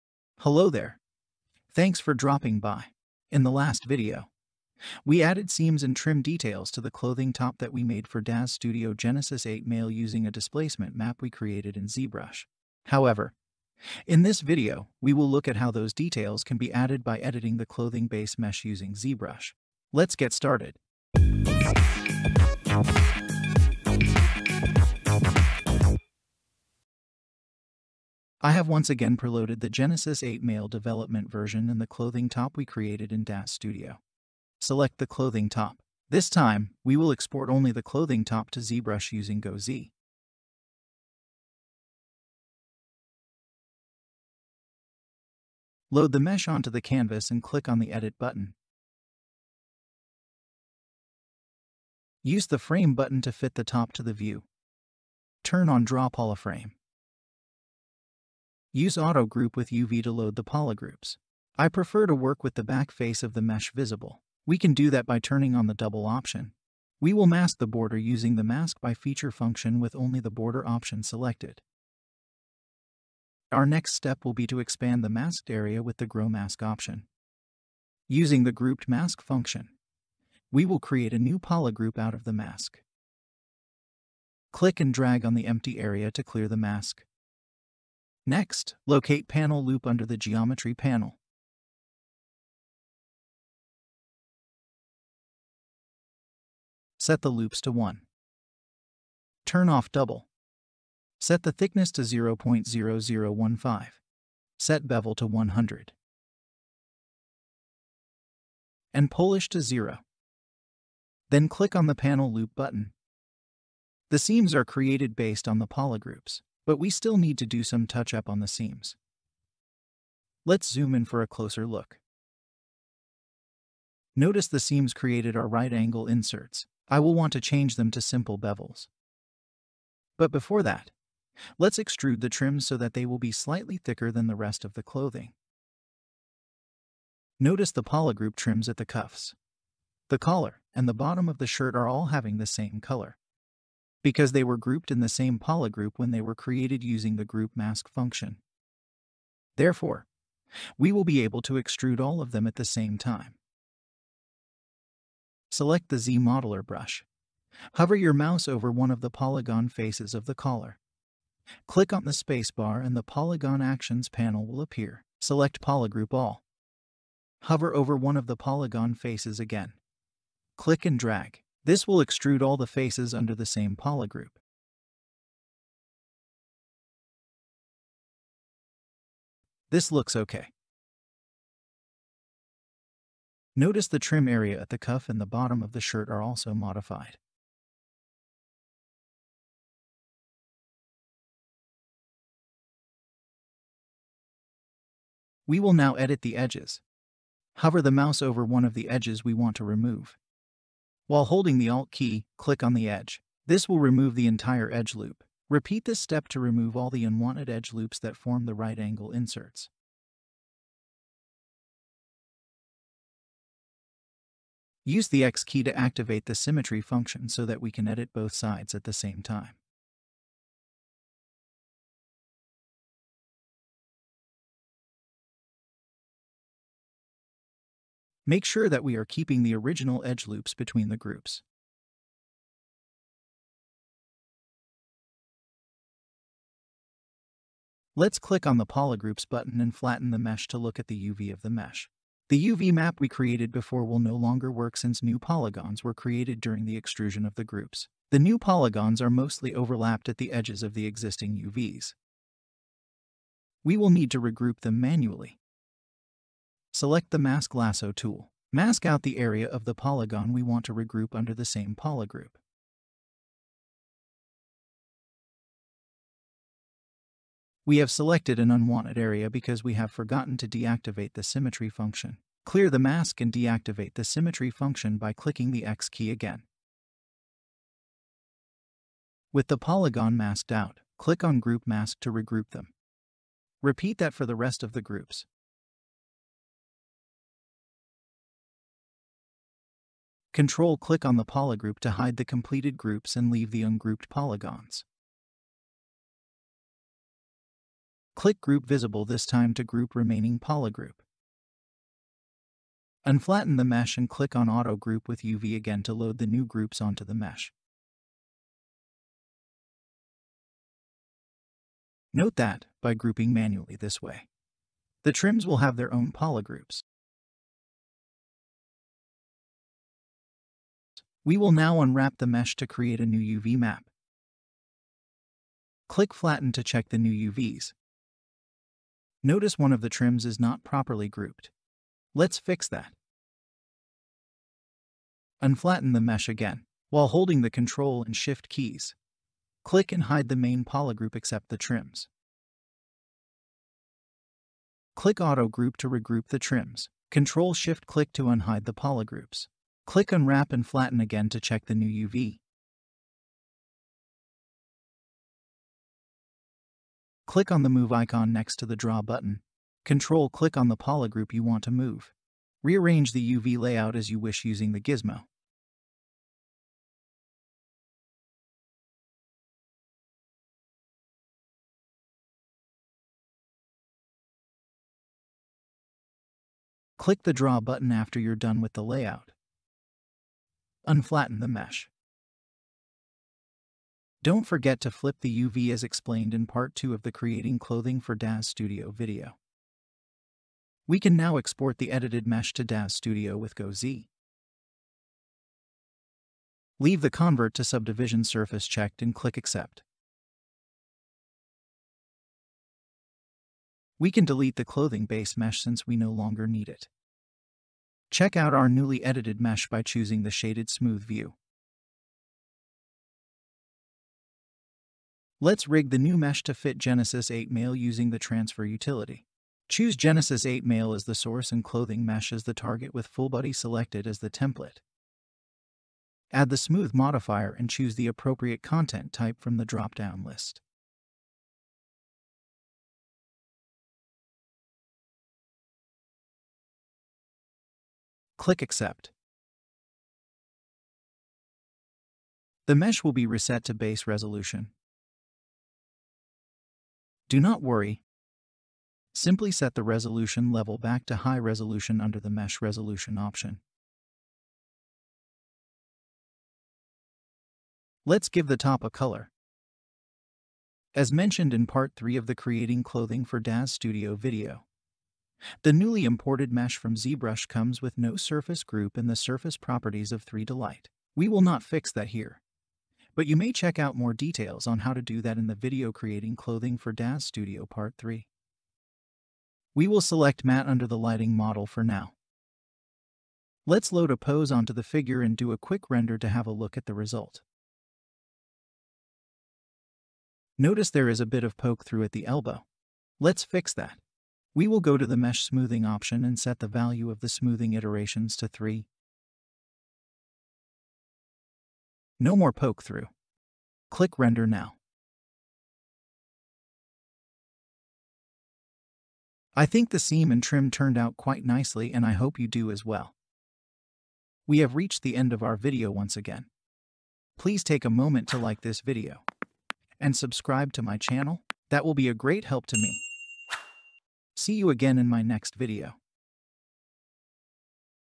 Adding detail to Daz Studio clothing in ZBrush
After looking at how to create clothing base for Daz Studio in our previous videos.

We will be looking at how to add seam details to the Daz Studio clothing base by editing the clothing mesh.

We'll see how we add hem and trims using some of the tools and brushes in Zbrush and bring it back to Daz Studio.